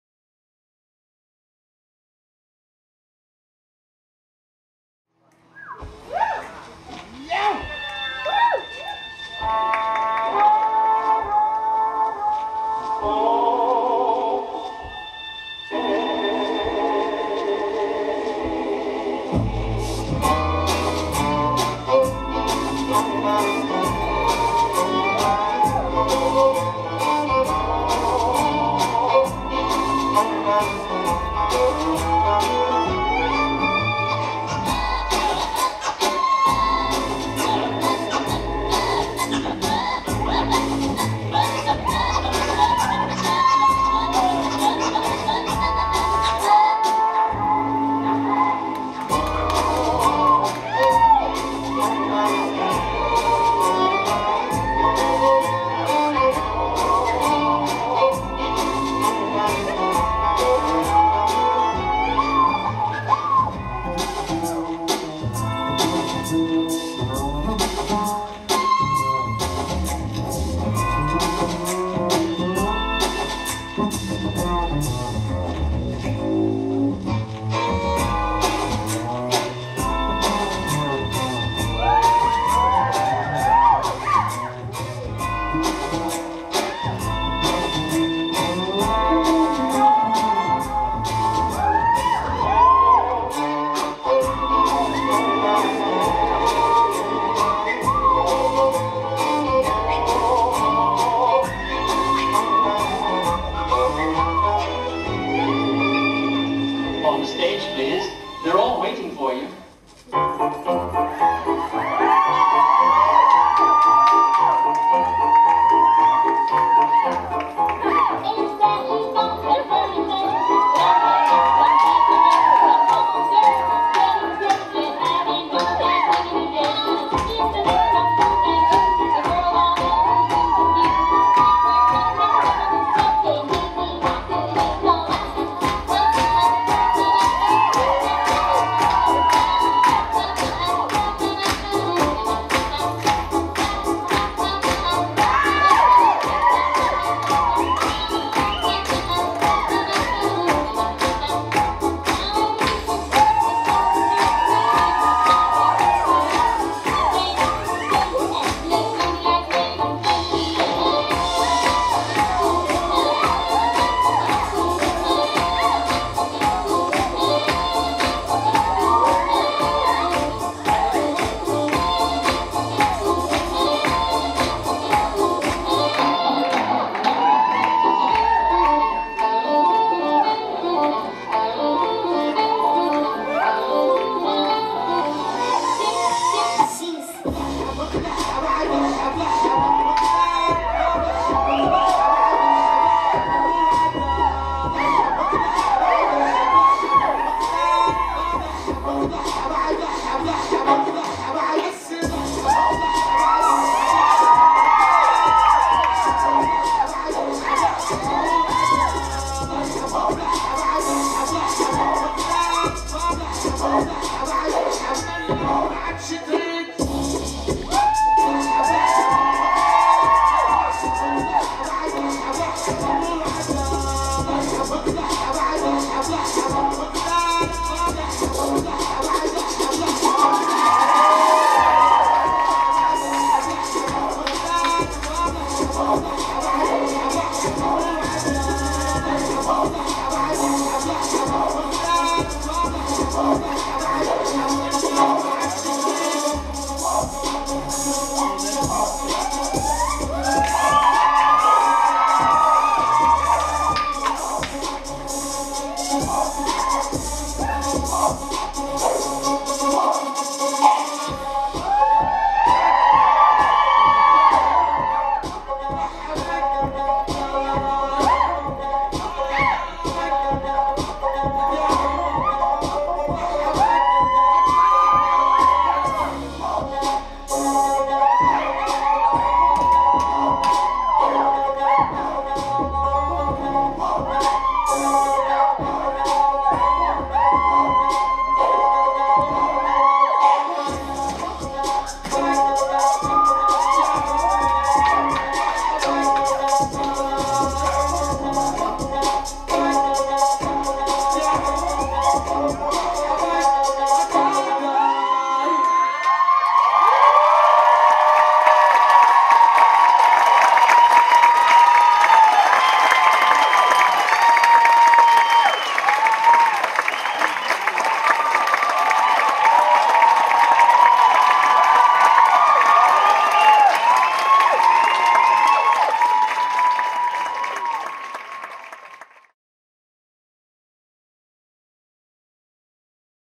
Celestial Bodies Belly Dance - perform "Lazy Showgirls"
Celestial Bodies Belly Dance performs at the 3rd annual Empire Burlesque Festival in Ithaca NY.